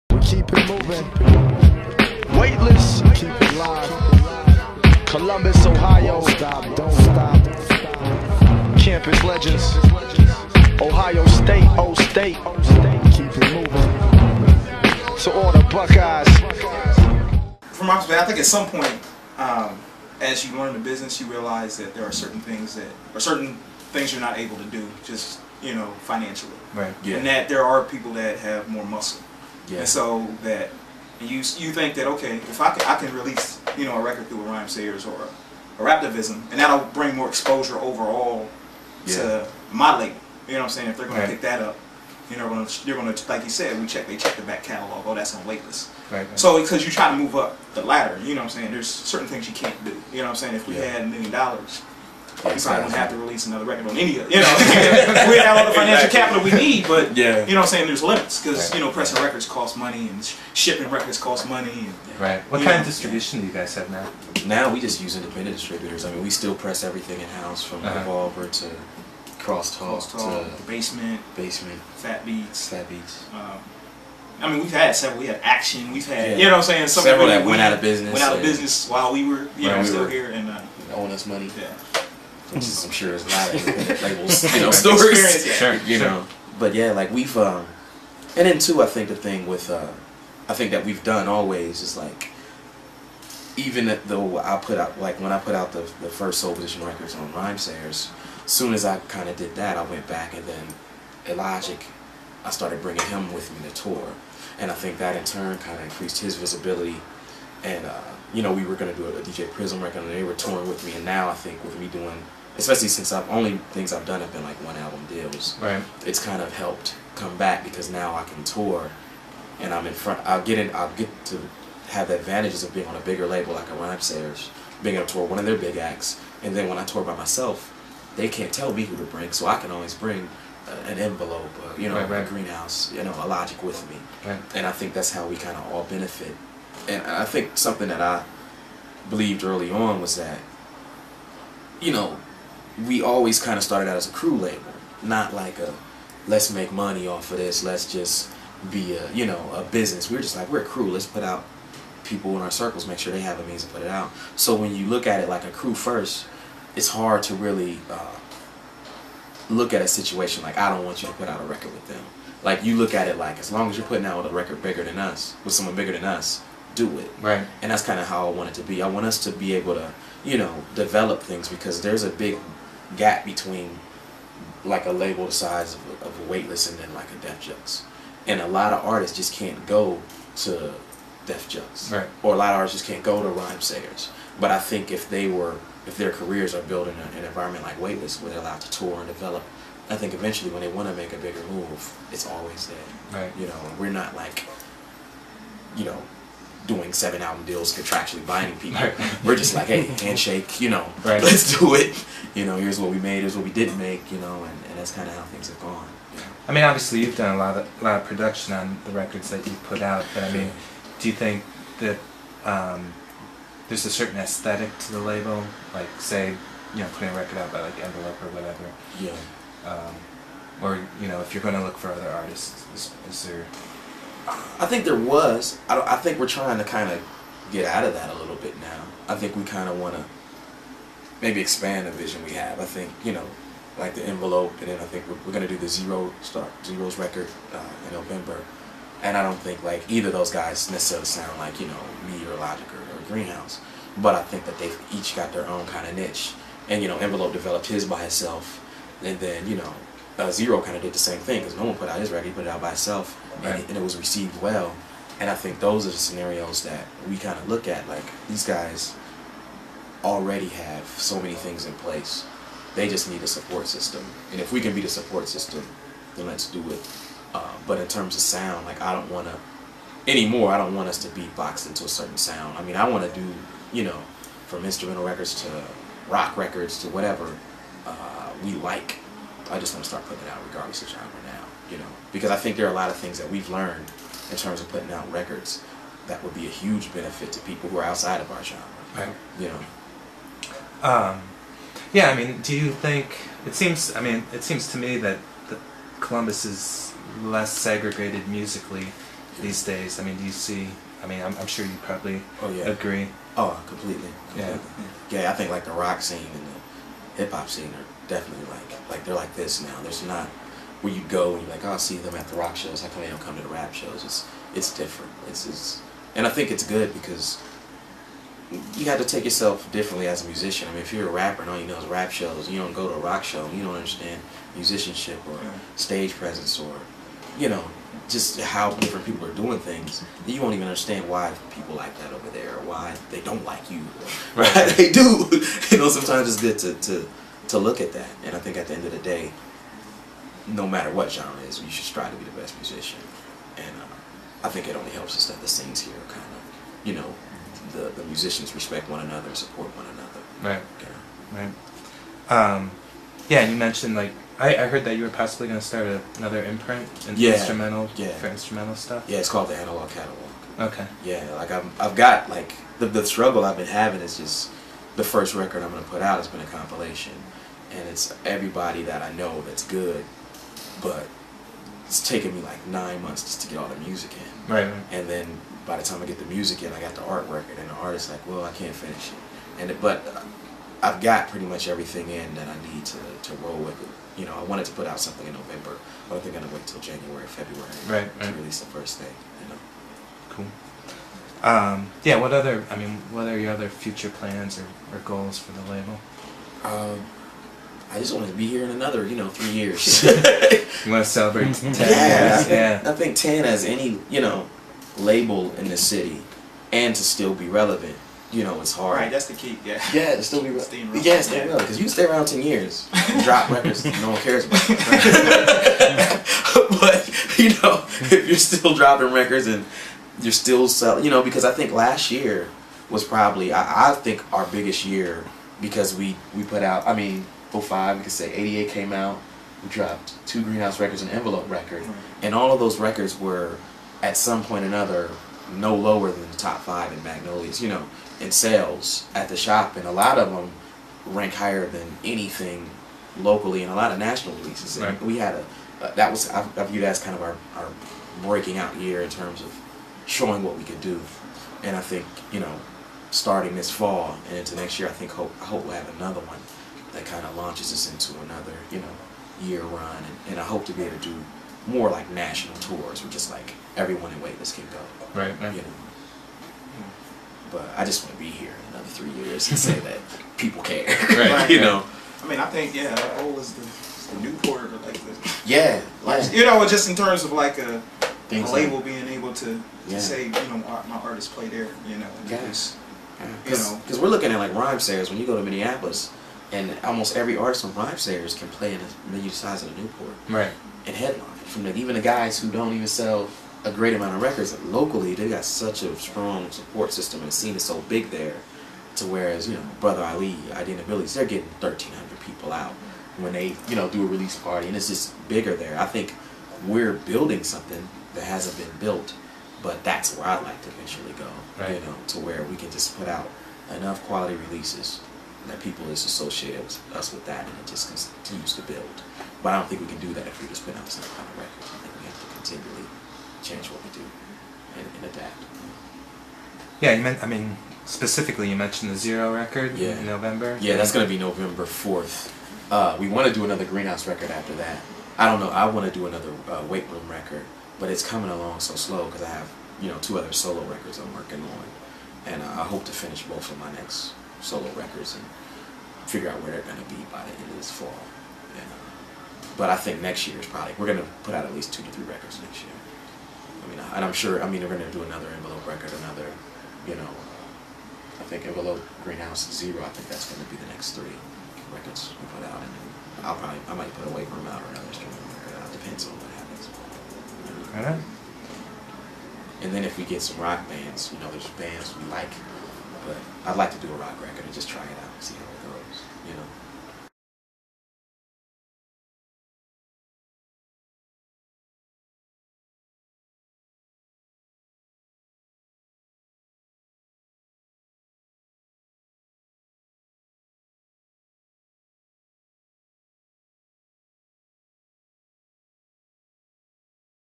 weColumbus Interviews Weightless Recordings (Part 2)
weColumbus.com interviews Manifest and Blueprint about their record label, Weightless Recordings.